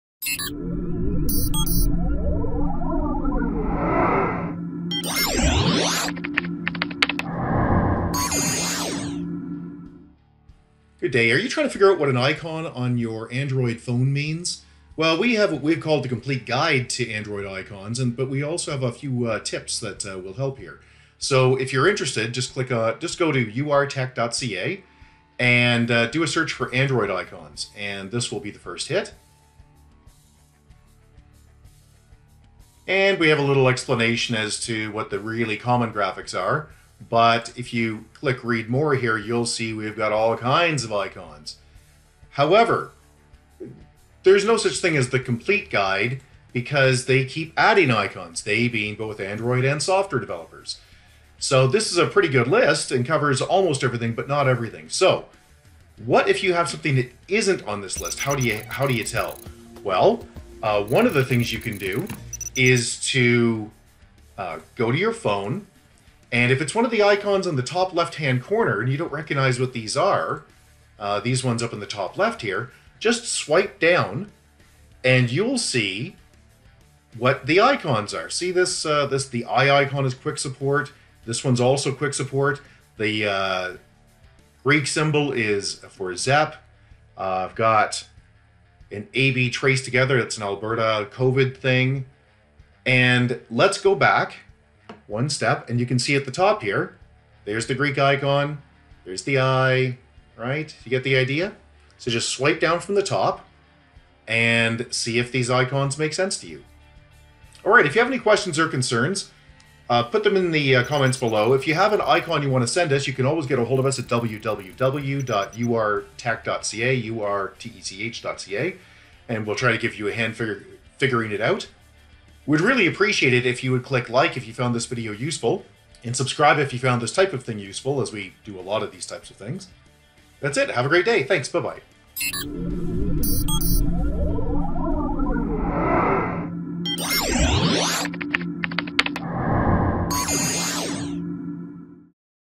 How to figure out What An Android Icon Represents
If you have an icon on your Android device and cannot figure out what it is, our list of Android Icons will likely be of help

However, new icons are added all the time am people often have no idea what they represent.  In this video we explain how to figure out what an android icon means.